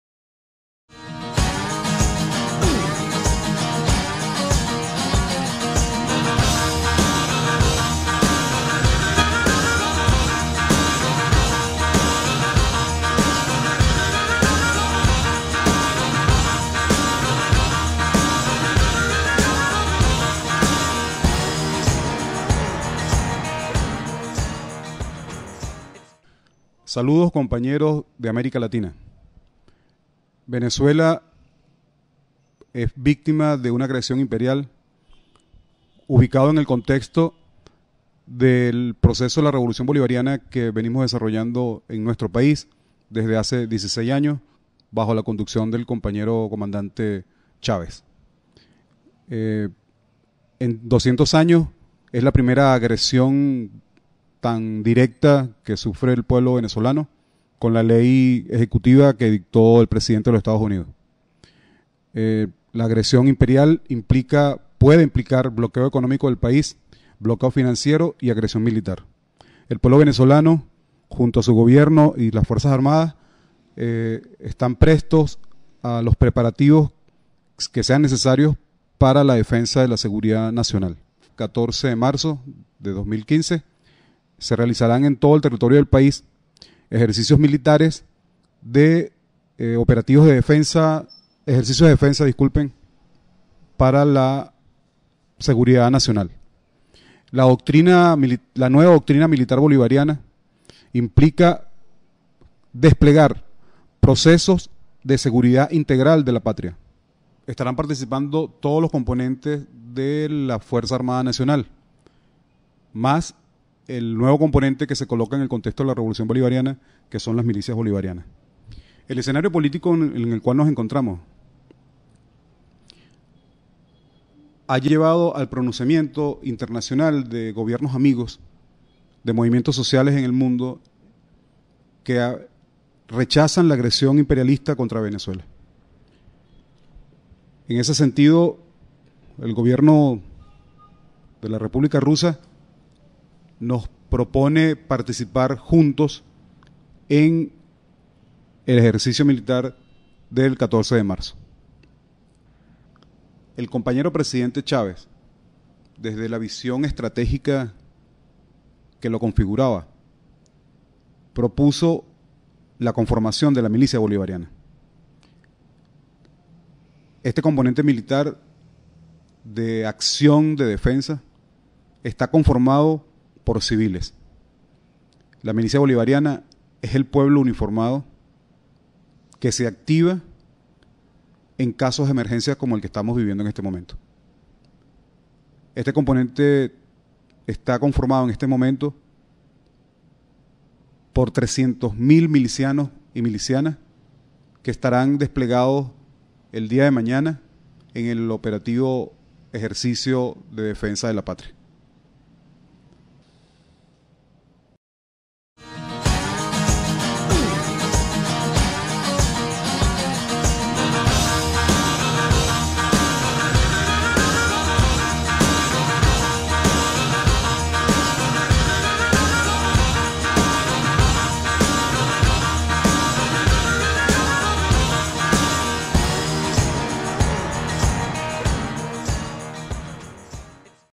Venezuela: "La doctrina militar bolivariana despliega procesos de seguridad integral de la patria"
Corresponsalía enviada desde Caracas, Venezuela.

Respondiendo al llamado para la protección de la soberanía nacional convocado por el presidente de la República, Nicolás Maduro; el pueblo participó las prácticas cívico-militares para definir los puntos estratégicos de defensa del pueblo, ante cualquier agresión por parte del imperialismo.
"La milicia bolivariana es el pueblo uniformado que se activa en casos de emergencia".